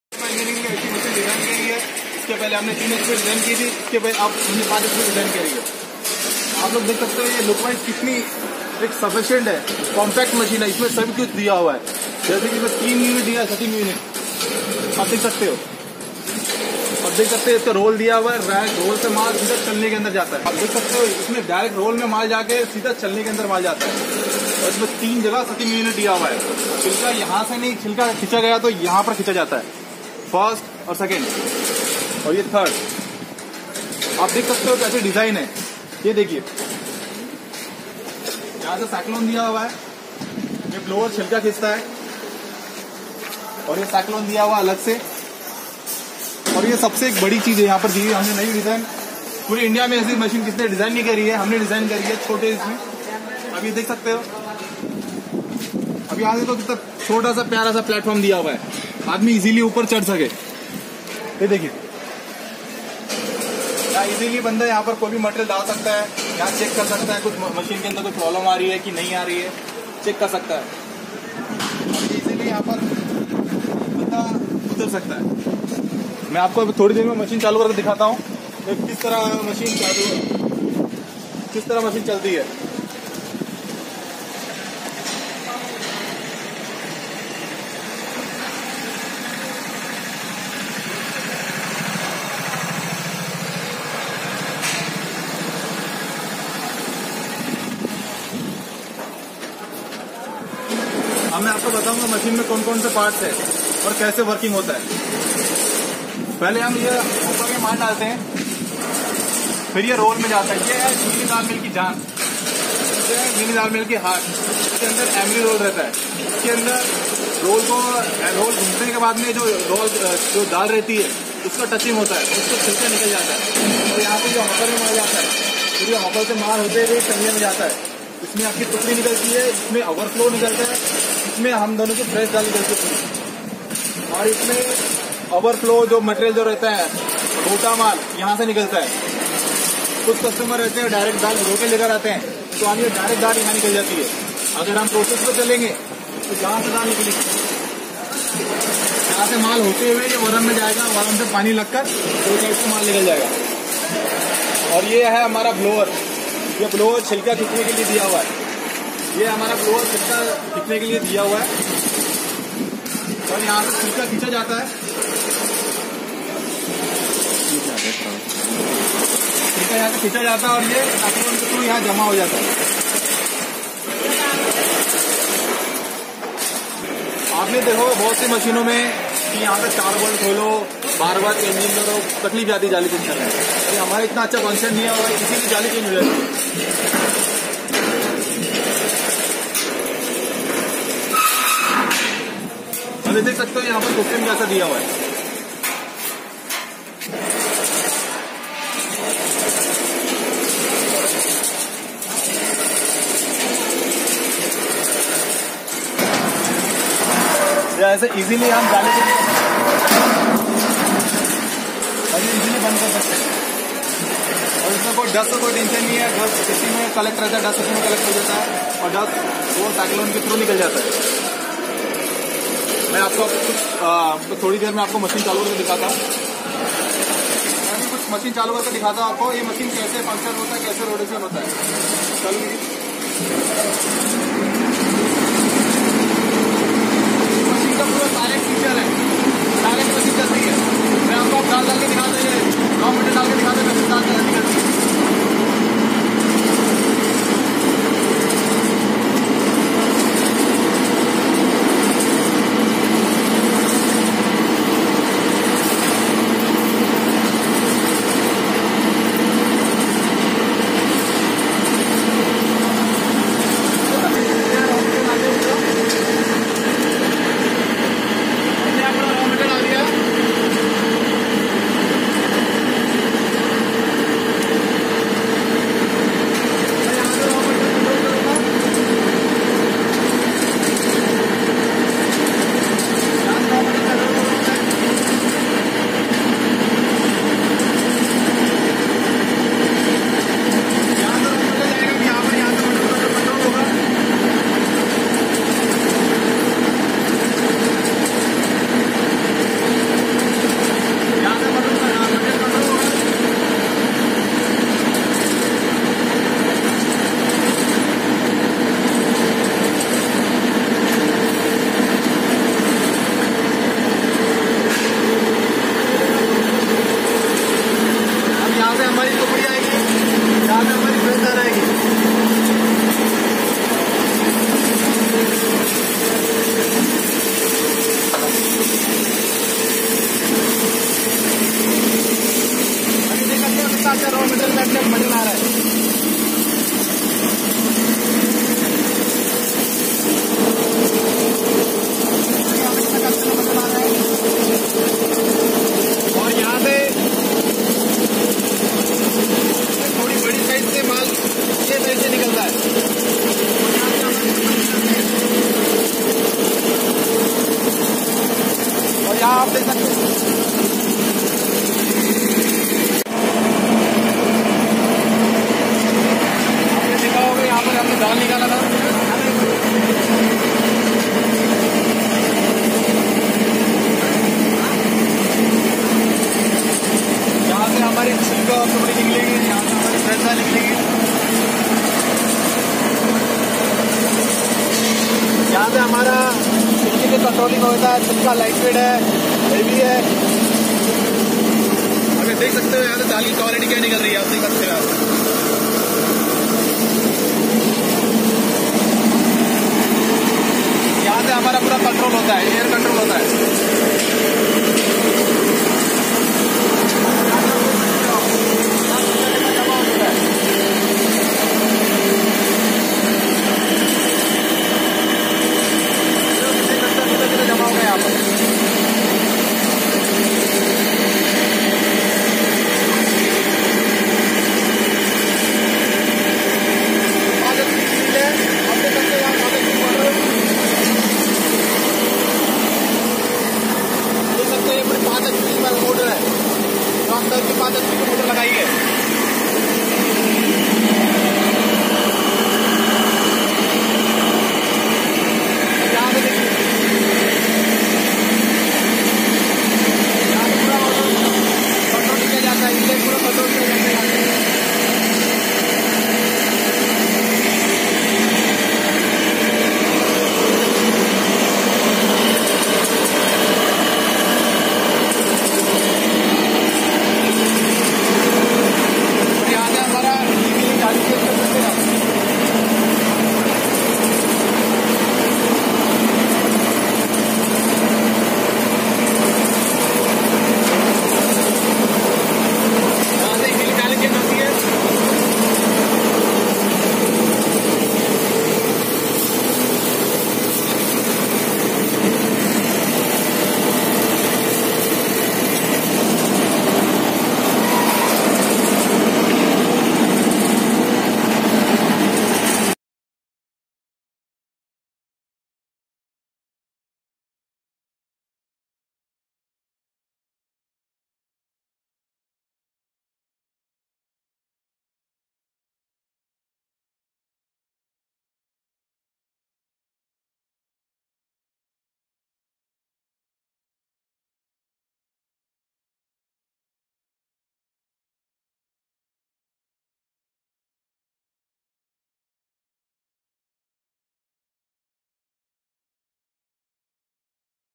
Mini dal mill latest dal mill advance dal mill hitechdalmill new business ©9370683288
Dal mill plant Deep Details & cost of project and working expanses

whatsaap no.  +91-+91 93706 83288
if you want more details you can text your name and city  on my whatsapp